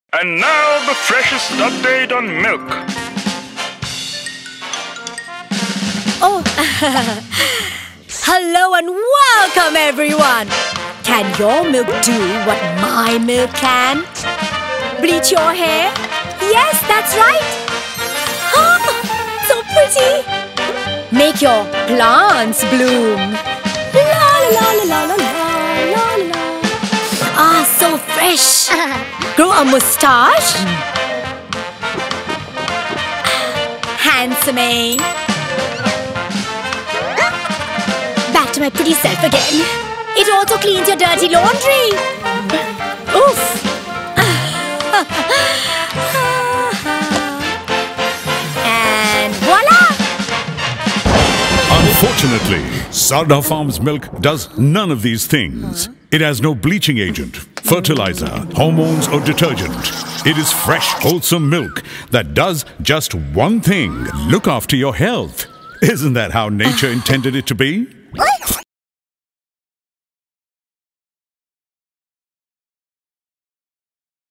Are you drinking milk, or detergent? | Sarda Farms
Are you drinking detergent? Urea? Or bleach? There is no control over what goes in your milk when it isn’t traceable. Some of the things added to increase the volume, whiten it, or make it last longer, make us shudder.

Every drop of milk coming from the Sarda Farms family farm is untouched by hands, till you do. Our own bottling and cold chain delivery system ensures we deliver fresh within hours of milking, just as nature intended it to be.